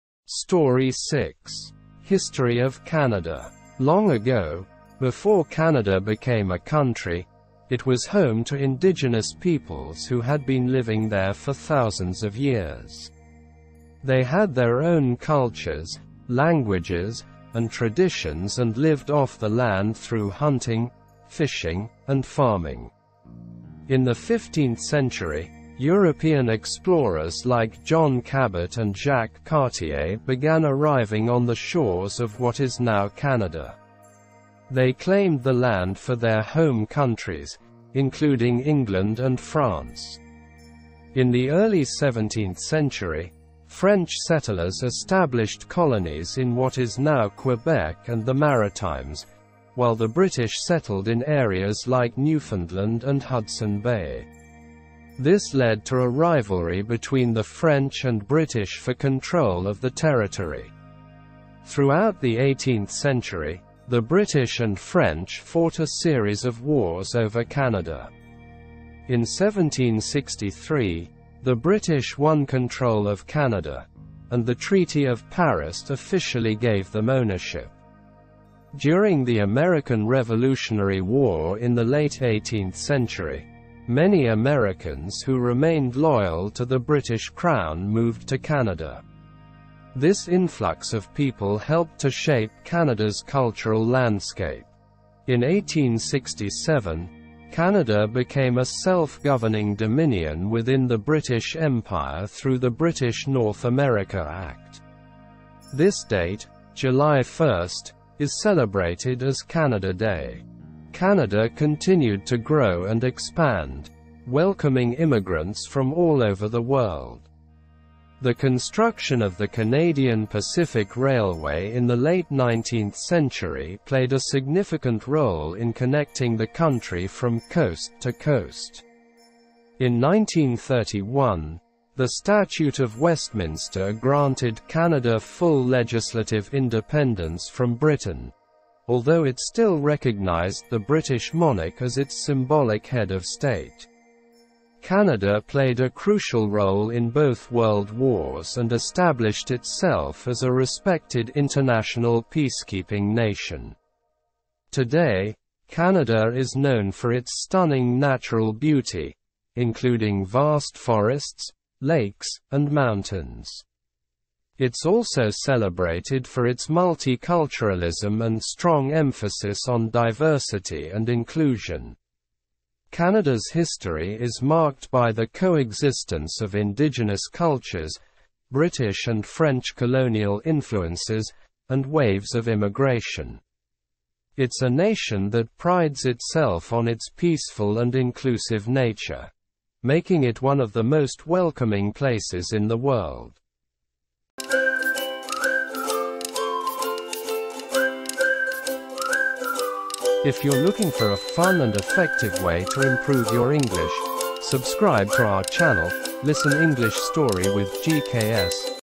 Learn English through Story Level 1 | History Of Canada | Beginner level | English speaking
englishlearning Learn English through Story Level 1 | History Of Canada | Beginner level | English speaking

This video is very helpful for those persons who really want to learn spoken English because here you can read and listen a very interesting short story. This short story name is " History Of Canada ".